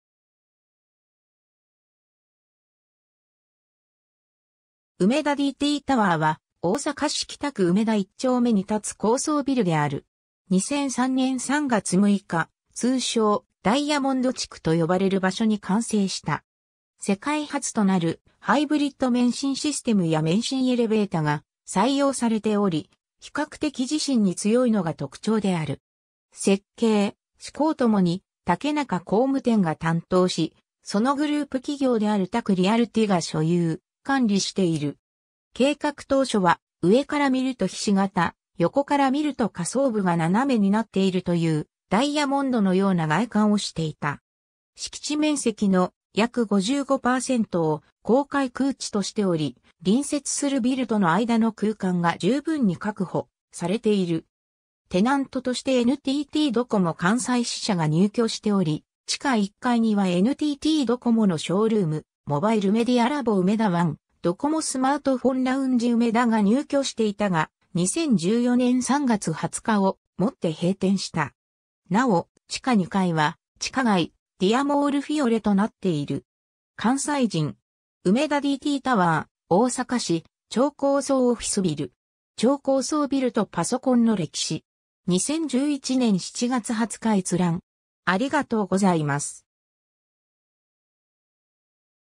梅田DTタワー
梅田DTタワー（うめだディーティータワー）は、大阪市北区梅田1丁目に建つ高層ビルである。
 2003年3月6日、通称「ダイヤモンド地区」と呼ばれる場所に完成した。
世界初となるハイブリッド免震システム（中間層免震）や免震エレベータが採用されており、比較的地震に強いのが特徴である。
 設計・施工共に竹中工務店が担当し、そのグループ企業であるTAKリアルティ（旧竹中不動産）が所有・管理している。
計画当初は、上から見ると菱形、横から見ると下層部が斜めになっているという、ダイヤモンドのような外観をしていた。
 敷地面積の約55%を公開空地としており、隣接するビルとの間の空間が十分に確保されている。
 テナントとしてNTTドコモ関西支社が入居しており、地下1階にはNTTドコモのショールーム「モバイルメディアラボ梅田+ONE」→「ドコモスマートフォンラウンジ梅田」が入居していたが、2014年3月20日をもって閉店した。
 なお、地下2階は地下街「ディアモール フィオレ」となっている。
 ^ ＠関西人. “梅田DTタワー 大阪市・超高層オフィスビル”. 超高層ビルとパソコンの歴史. 2011年7月20日閲覧。